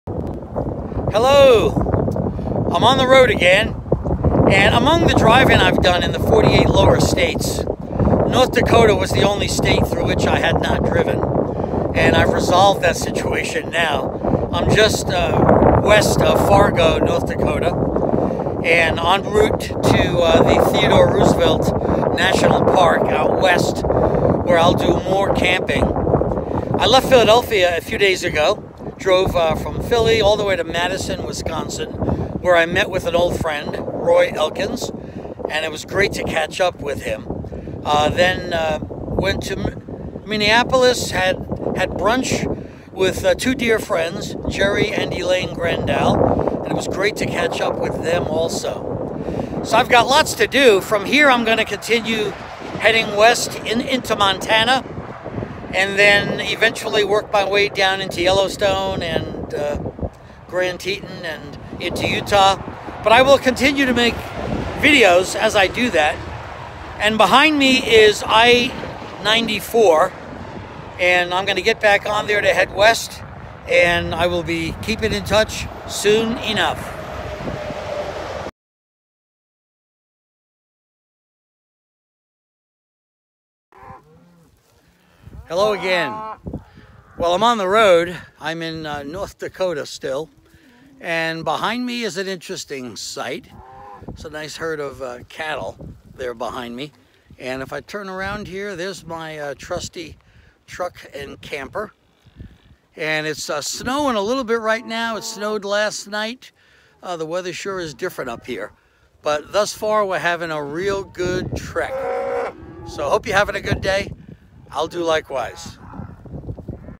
01 North Dakota - Starting Here
Getting started on a westbound trek. Cattle herd and visits with friends are featured here.